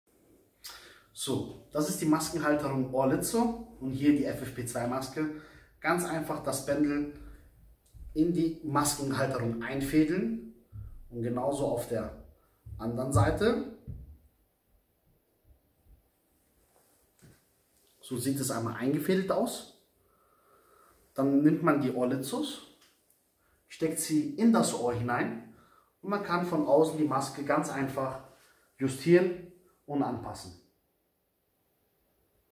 Maskenhalterung „Ohrlizzo“
Friseurmeister Enzo Olizzo mit einer Erfindung, die  nicht nur sein Handwerk erleichtert.